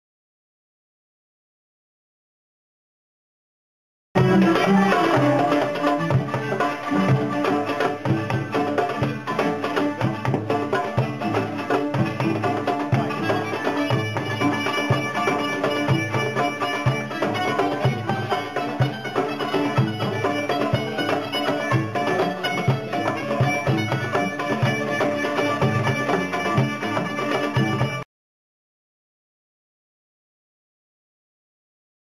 Gilded Serpent reports- The Kervan Ensemble at PPM, SF
May 10, 2008, Pena Pachamama Restaurant in North Beach San Francisco.
The Kervan Ensemble, a local Turkish band, warms up the crowd before George Lammam's band comes on.
Kervan members:Halik Kecelioglu, Eliot bates, Ahmet Cegin, Sean Turgis, Samuel Foster.